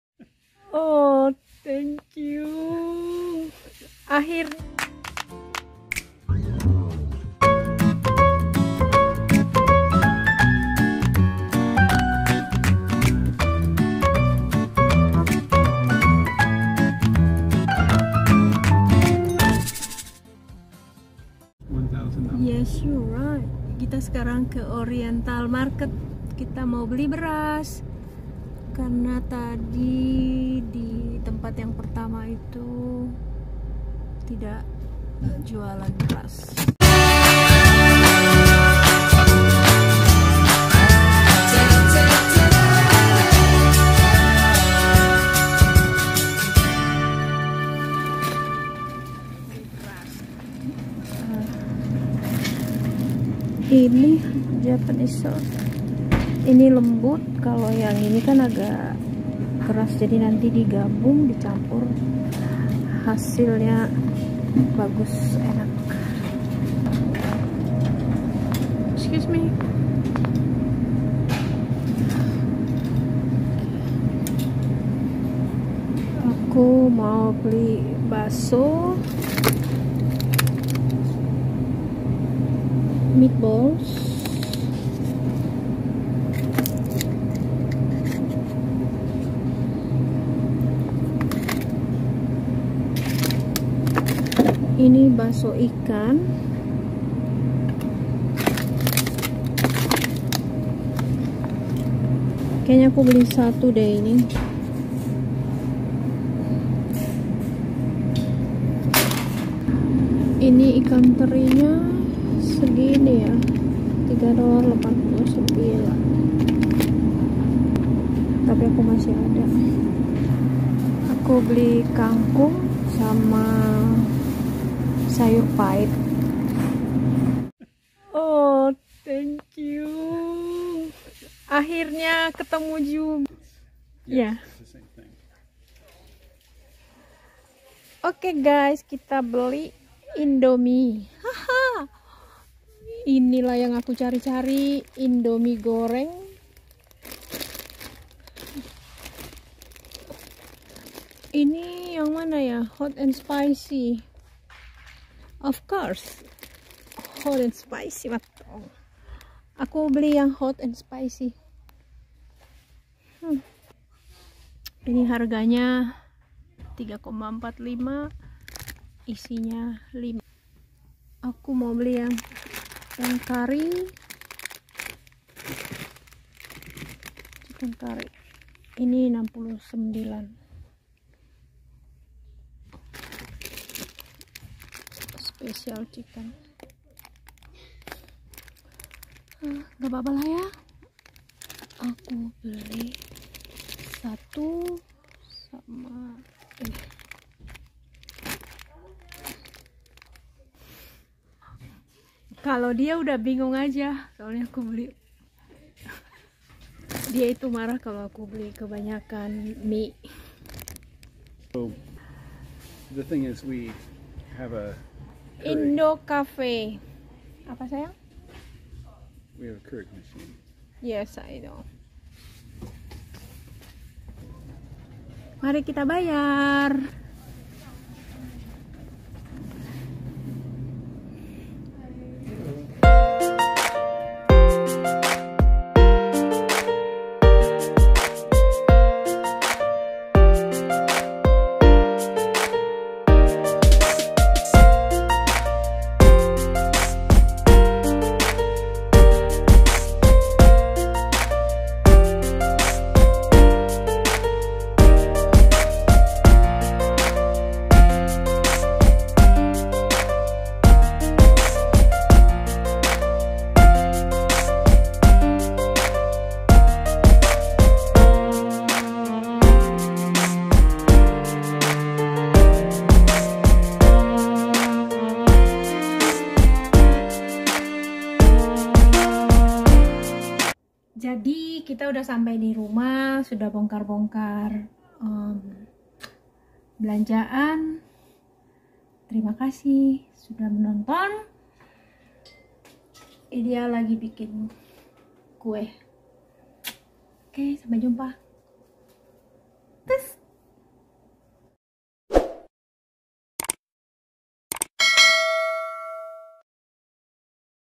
Bosan makanan western || Belanja di Toko Asia
Video kali ini Belanja kebutuhan dapur😘 karena Jumat pas libur arbour day.

Kami belanja ke Toko Asia 2 minggu sekali,
Lumayan ada yang saya cari.

Smoga bisa menghibur kalian ya my Friends❤

SUPAYA KAMI MAKIN SEMANGAT UTK BIKIN VIDEO .
💌KALAU ADA SARAN BISA DIKASIH MENTION YA..💟